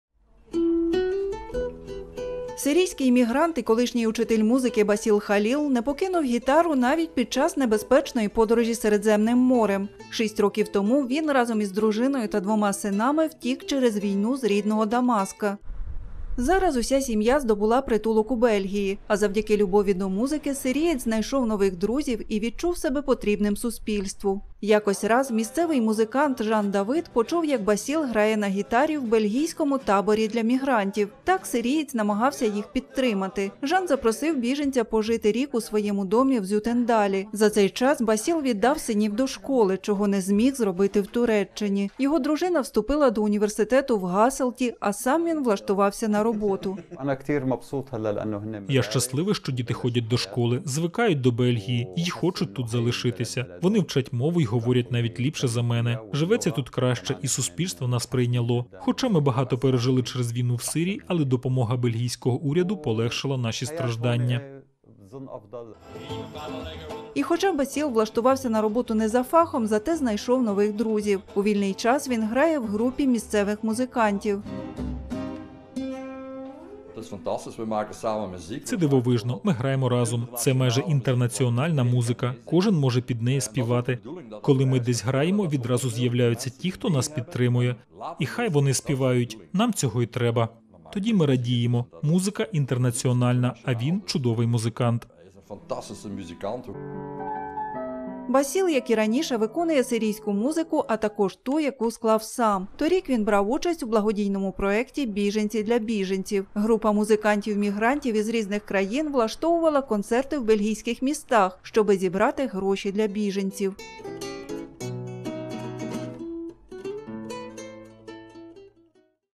Любов до музики допомогла сирійському мігрантові знайти друзів
Сирійський мігрант і колишній учитель музики Басіл Халіл не покинув гітару навіть під час небезпечної подорожі Середземним морем. Шість років тому він разом із дружиною та двома синами втік через війну з рідного Дамаска.
Зараз уся сім'я здобула притулок у Бельгії. А завдяки любові до музики сирієць знайшов нових друзів і відчув себе потрібним суспільству.